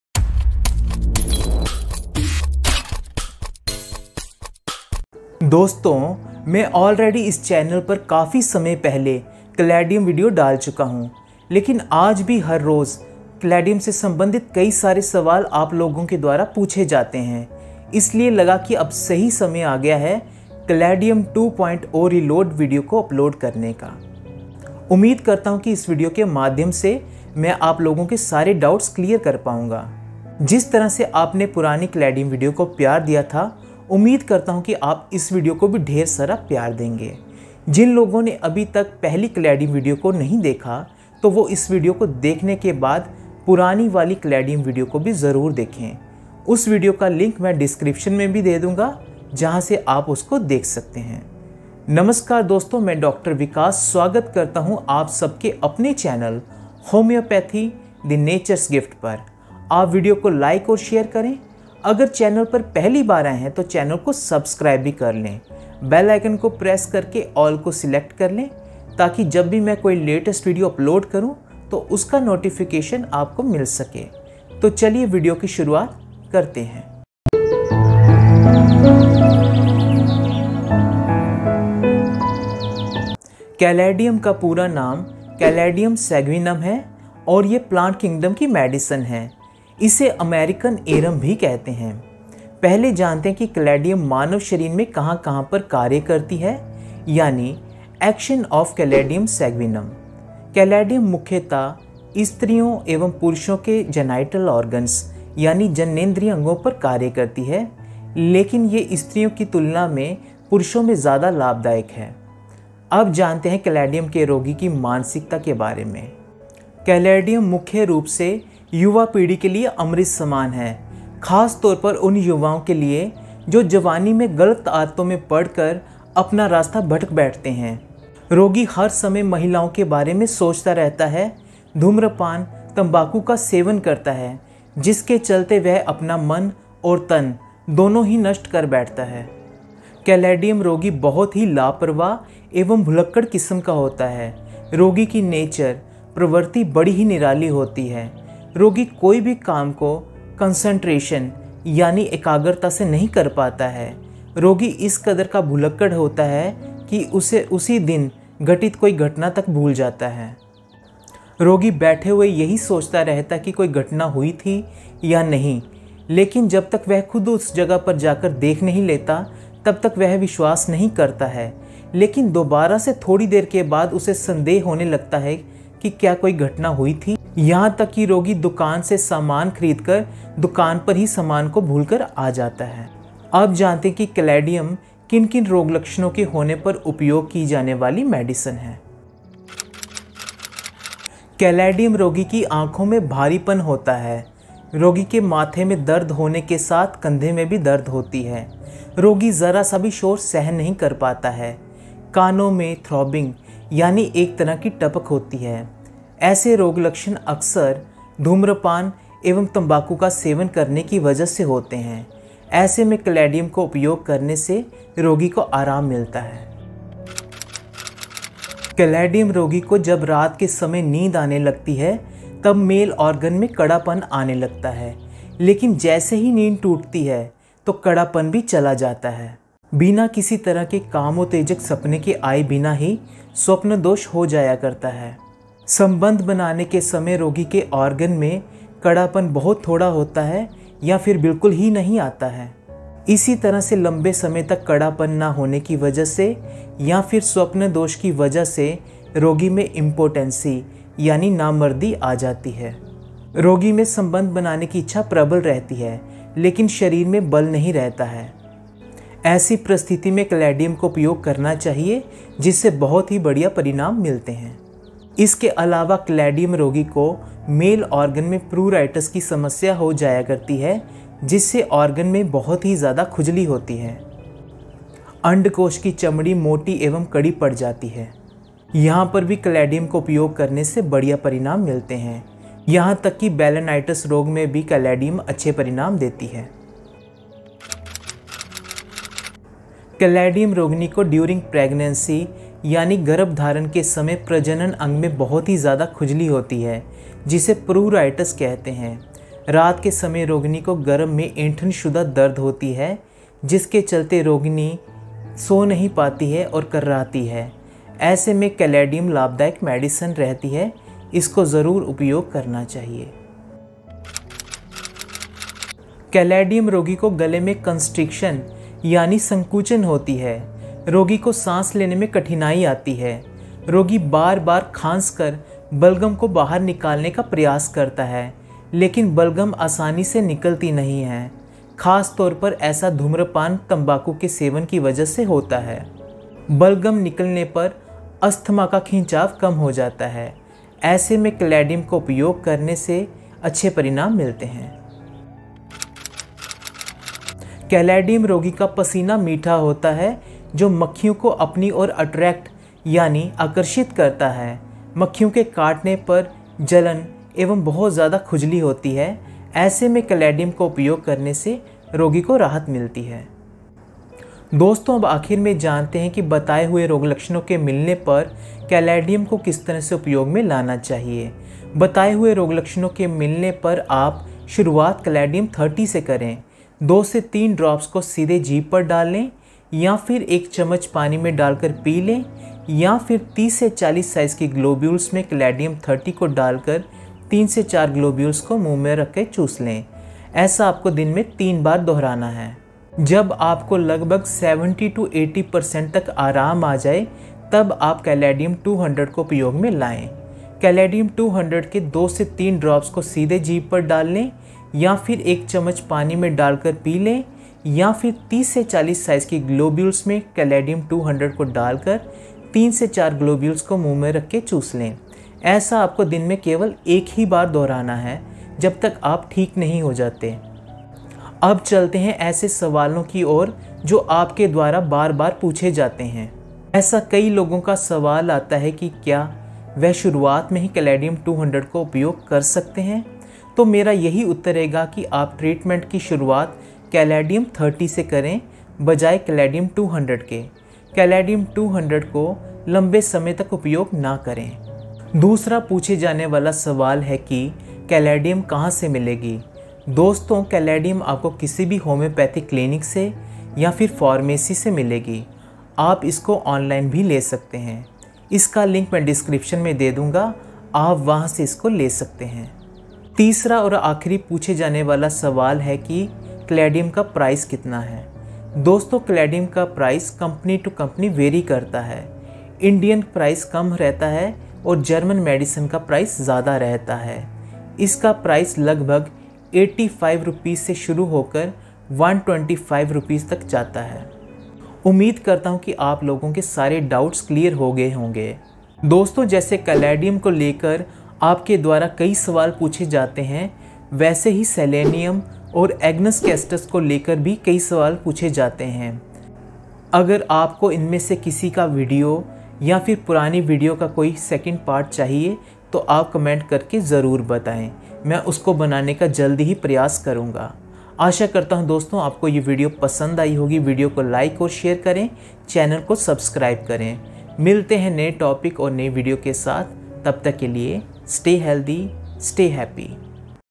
Caladium Seguinum 30 In Hindi II Caladium Homeopathic Medicine II Caladium 30 II Caladium
Dose :
Take 2-3 drops of Caladium Seguinum 30 directly on tongue three times a day.

Old Caladium video.

To buy, SBL Caladium 30.

To buy, Dr. Reckeweg's Caladium  30.